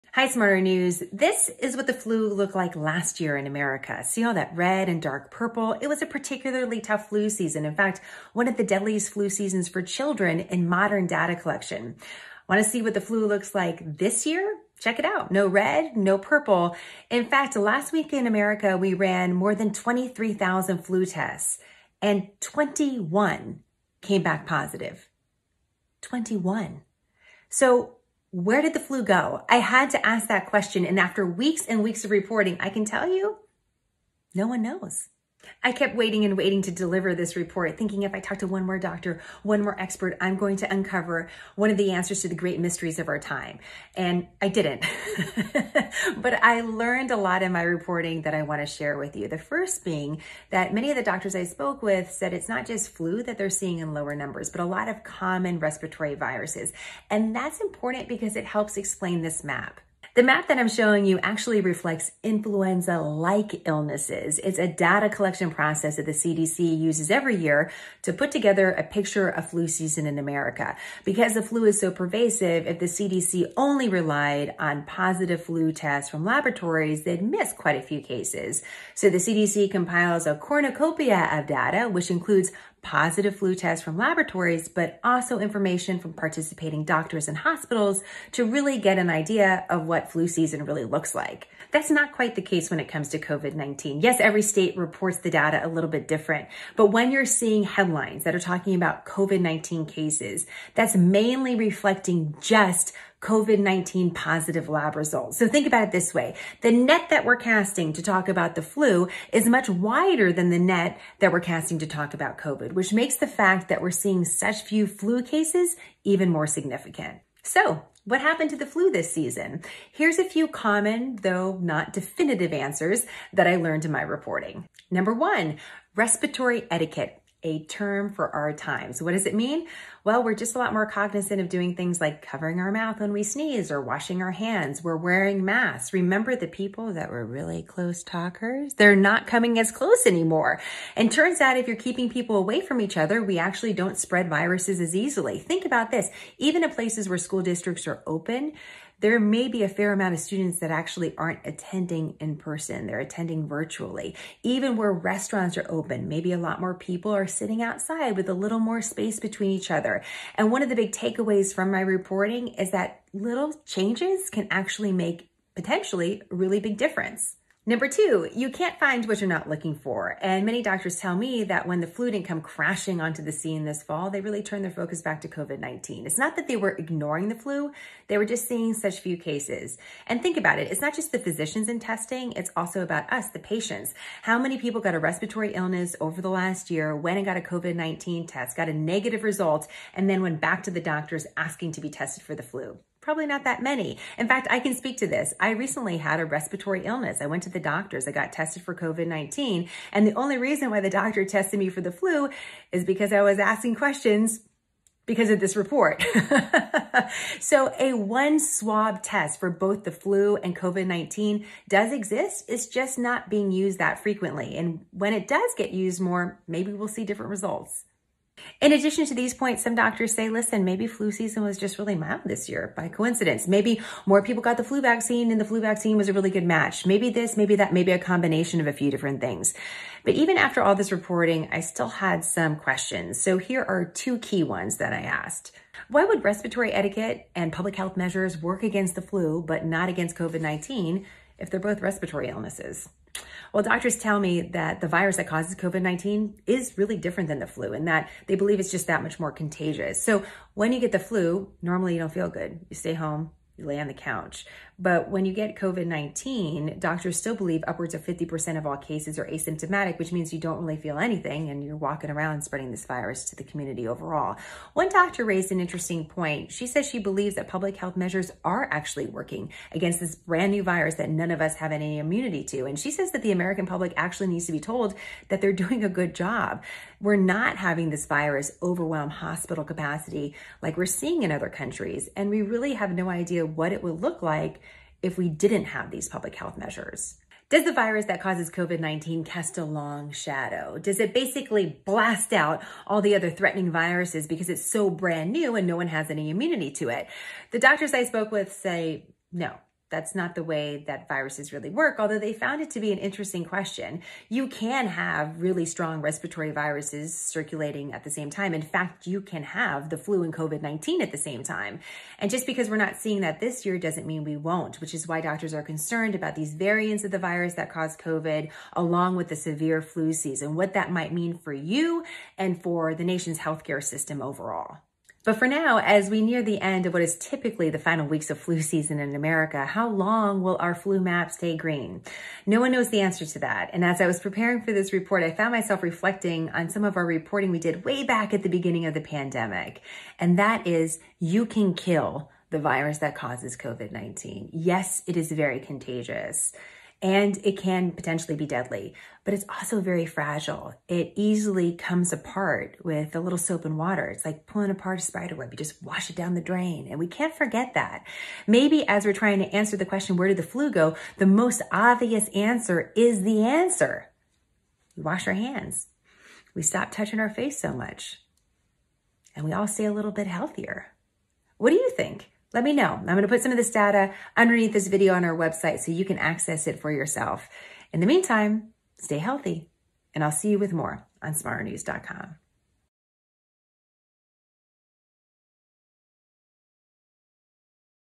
Where did the flu go?
What happened to the other respiratory virus we use to talk so much about? Here’s some important context for discussing the flu during the Covid-19 pandemic and what doctors think is happening to influenza in America.
What do you think is the most likely case?